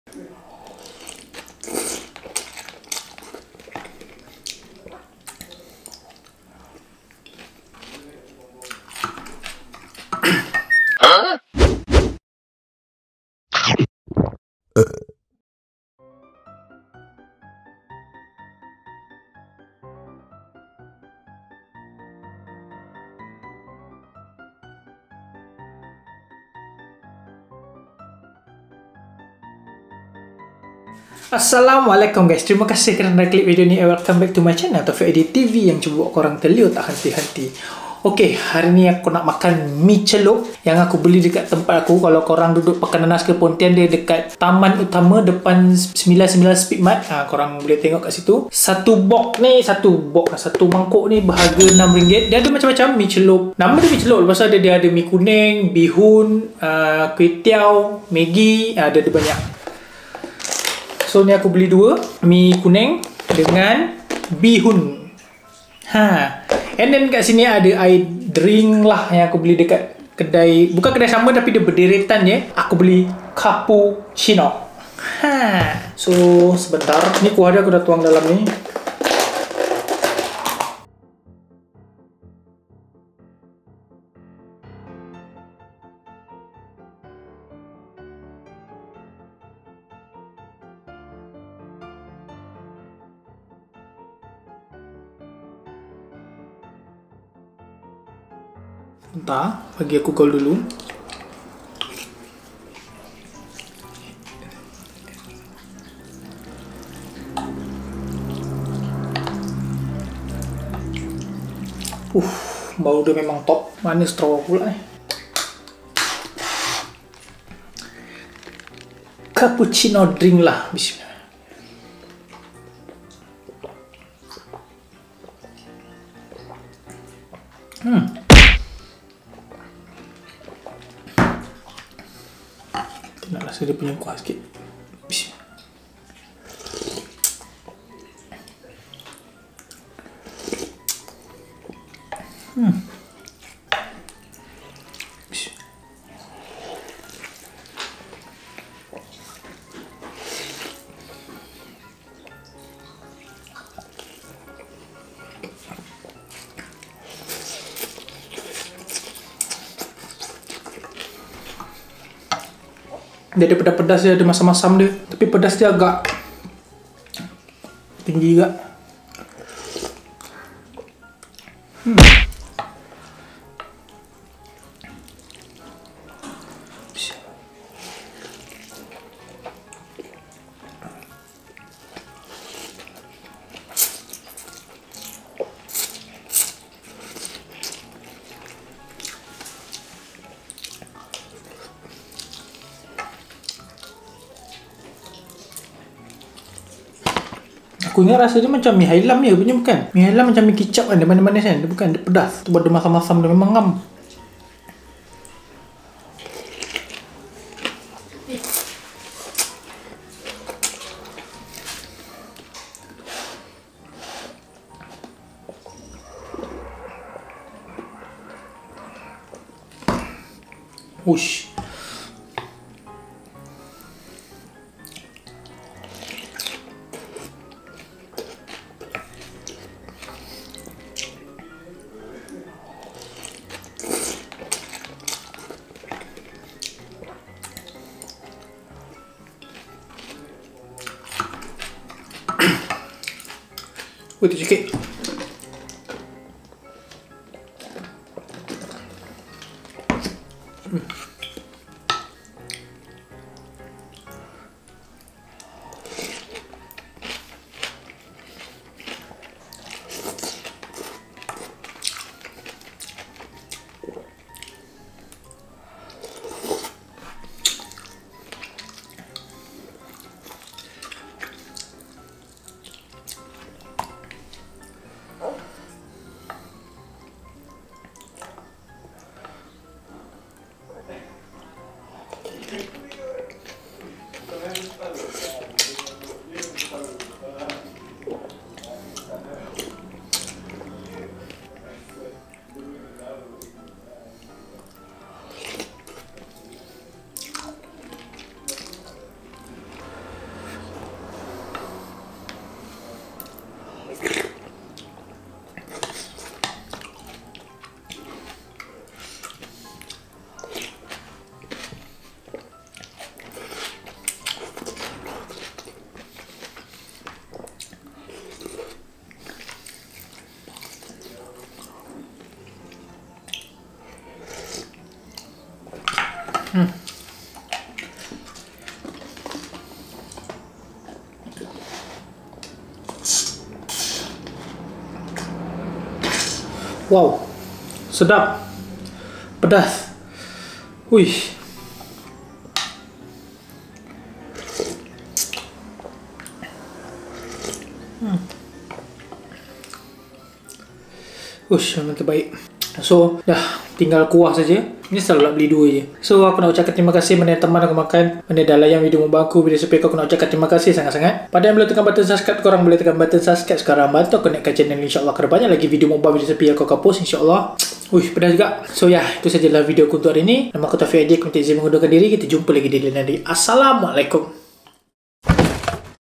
Mee CELUP terpadu dekat tempat aku (mukbang malaysia)
Haaa memang padu dia punya kick mee celup ni sampai terkulup lidah aku nak makan tu, padu teruk

 ▬▬ Follow Socmed Kitaorang  ▬▬

▬▬  ▶︎ Hashtag ◀︎  ▬▬